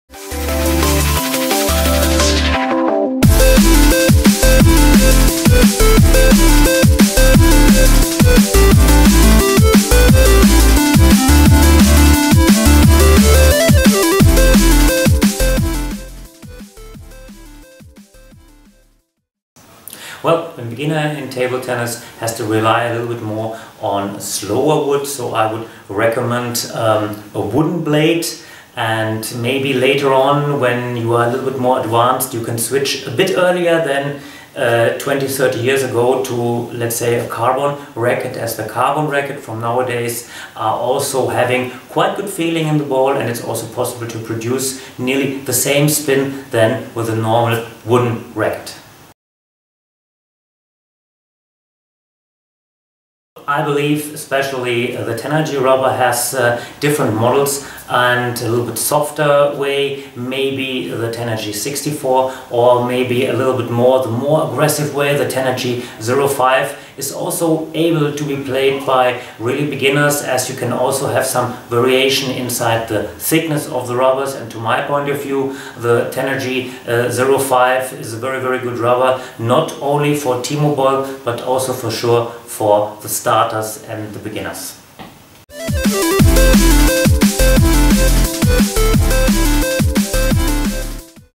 Table Tennis - Ask the Coach with Richard Prause - Part 17 Beginner's equipment
Ask the Coach - Richard Prause answers your questions. You have asked a lot of questions and now we publish Richard's answers!

In this part Richard gives some advice which equipment might be useful for a beginner.